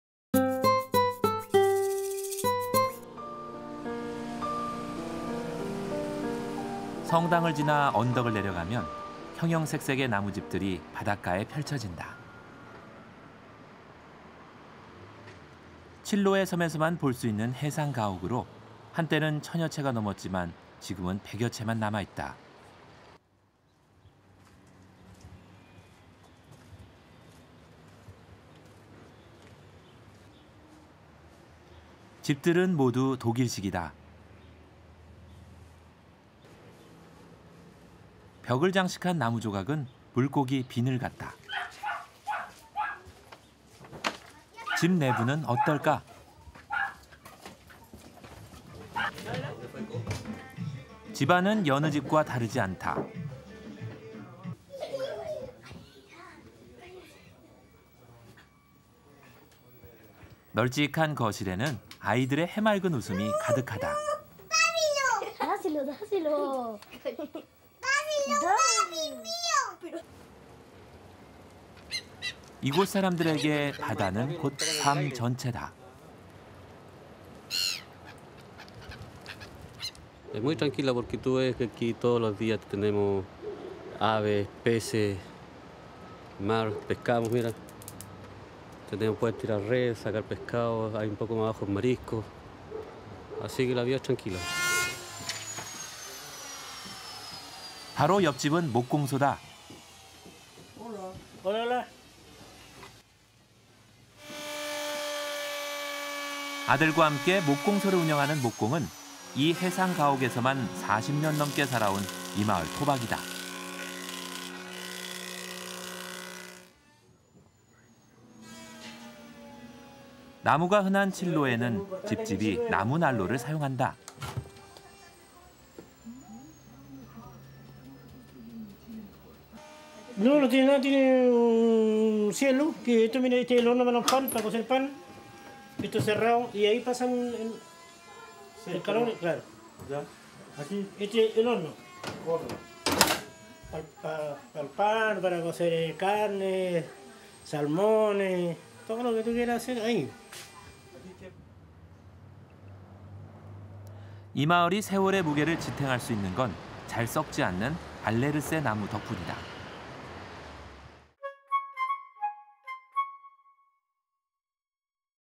【K】Chile Travel-Los Lagos[칠레 여행-로스라고스]칠로에 명물, 카스트로 수상가옥/Chiloe Castro Floating Village/Island/Sea
[한국어 정보]
성당을 지나 언덕을 내려가면 형형색색의 나무집들이 바닷가에 펼쳐진다. 칠로에 섬에서만 볼 수 있는 해상가옥으로 한 때는 천여 채가 넘었지만 지금은 백여 채만 남아있다. 집들은 모두 독일식이다. 벽을 장식한 나무 조각은 물고기 비늘 같다. 집 내부는 어떨까? 집안은 여느 집과 다르지 않다. 널찍한 거실에는 아이들의 해맑은 웃음이 가득하다. 이곳 사람들에게 바다는 곧 삶 전체다. “매일 새, 물고기들과 함께 할 수 있는 바다가 있어 좋아요. 집 밑의 바다로 내려가면 바로 조개도 잡을 수 있고 이곳이 평온하고 행복하죠.“ 바로 옆집은 목공소다. 아들과 함께 목공소를 운영하는 목공은 해상가옥에서만 40년 넘게 살아온 이 마을 토박이다. 나무가 흔한 칠로에는 집집이 나무 난로를 사용한다. “이 난로는 특별하지는 않지만 이 마을 전통이에요. 빵을 굽거나 요리할 때 사용하는데 여기를 닫고 열기가 위로 지나가는 일종의 오븐이죠. 고기나 연어들을 굽기도 해요.” 이 마을이 세월의 무게를 지탱할 수 있는 건 잘 썩지 않는 알레르세 나무 덕분이다.

Go down the hill past the cathedral unfolds on the beach colorful wooden houses. When a house with a sea view can only fill a thousand islands in a while, but now has left more than a hundred chaeman. The houses are all German. The walls are decorated with pieces of wood like fish scales. What about inside the house? The house is not much different from any other house. The spacious living room is filled with children's laughter haemaleun. This sea is full of people to life soon. "I love the sea can be with every new fish. Go down to the house under the sea can also catch shellfish and right here I have peace and happiness. "It is right next door to the carpenter shop. Woodworking running a carpentry shop with his son maritime village houses only is this native has lived for more than 40 years. The tree is common to use a door-to-door fills the wood stove. "This stove is the village's traditional but not special. Bake the bread's sort of oven heat when cooking or passing close over here for use. I'm also burning the meat or salmon. "This town is able to support the weight of years, it s well rotted wood that is thanks to Alessandro reuse.

Ir abajo de la colina más allá de la catedral se desarrolla en la playa coloridas casas de madera. Cuando una casa con vistas al mar sólo puede llenar un millar de islas en un tiempo, pero ahora ha dejado más de un centenar de chaeman. Las casas son todas alemán. Las paredes están decoradas con piezas de madera como escamas de pescado. ¿Qué hay dentro de la casa? La casa no es muy diferente de cualquier otra casa. El amplio salón está lleno de haemaleun risa de los niños. Este mar está lleno de gente a la vida pronto. "Me encanta el mar puede ser con cada nuevo pez. Desciende a la casa bajo el mar también puede coger mariscos y aquí tengo la paz y la felicidad." Está justo al lado del taller de carpintería. Carpintería funcionamiento de un taller de carpintería con su hijo casas de pueblo marítimo sólo es nativa ha vivido por más de 40 años. El árbol es común el uso de una puerta de puerta a puerta, llena la estufa de leña. "Esta estufa es de la aldea tradicional, pero no es nada especial. Hornear especie de pan de calor del horno al cocinar o pasar cerca de aquí para su uso. También me estoy quemando la carne o salmón." Esta ciudad es capaz de soportar el peso de los años, que s bien madera podrida que es gracias a la reutilización de Alessandro.

[Information]
■클립명: 남미11-칠레03-05 칠로에 명물, 카스트로 수상가옥/Chiloe Castro Floating Village/Island/Sea
■여행, 촬영, 편집, 원고: 이상익 PD (travel, filming, editing, writing: KBS TV Producer)
■촬영일자: 2012년 2월 February

[Keywords]
남미,America,아메리카,칠레,Chile,이상익,2012,2월 February,로스라고스,Los Lagos